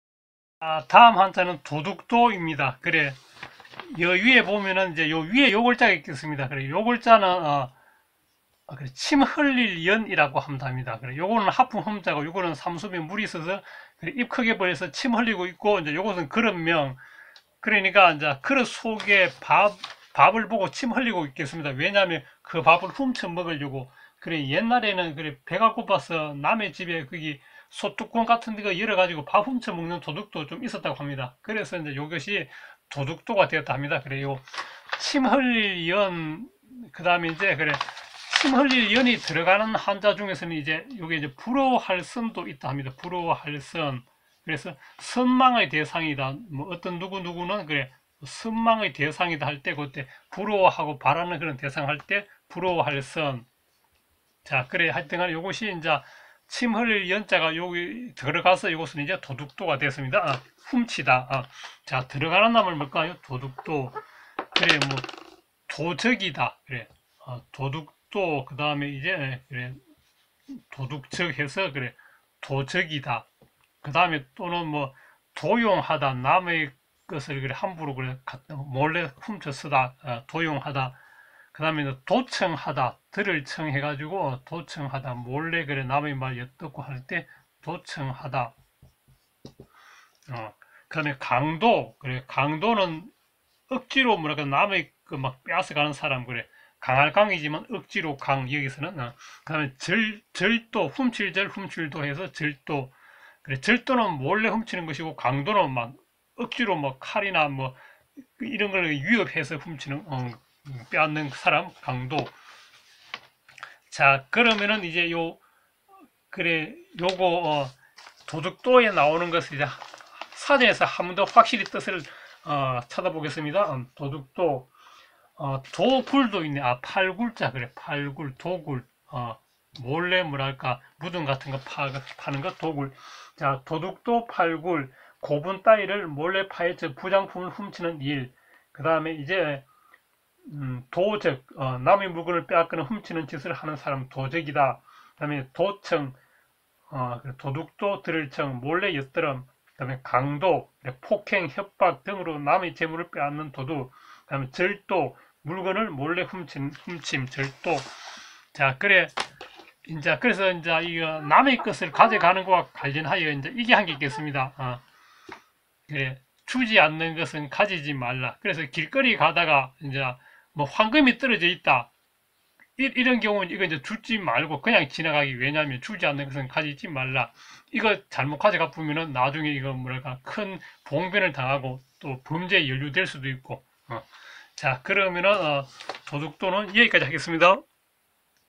4급 배정한자 60 : 盜 도둑 도
盜 “도둑 도”
* 침흘릴 연 + 그릇 명. (참고: ‘침 흘릴 연’이 들어가서 음의 역할을 한 글자는 ‘부러워할 선(羨望선망). 그릇 속(의 음식)을 보고 사람이 입을 크게 벌려서 침을 흘린다, 옛날에는 배가 고파 밥을 훔쳐가는 도둑이 많았다고 한다.
* 盜賊도적. 盜聽도청-몰래 엿듣는 일. 强盜강도. 捕盜廳포도청. 盜難도난-도둑을 맞는(또는 물건을 도둑맞는) 재난. 盜用도용-남의 것을 몰래 훔쳐 씀. 盜掘도굴. 竊盜절도-물건을 몰래 훔침.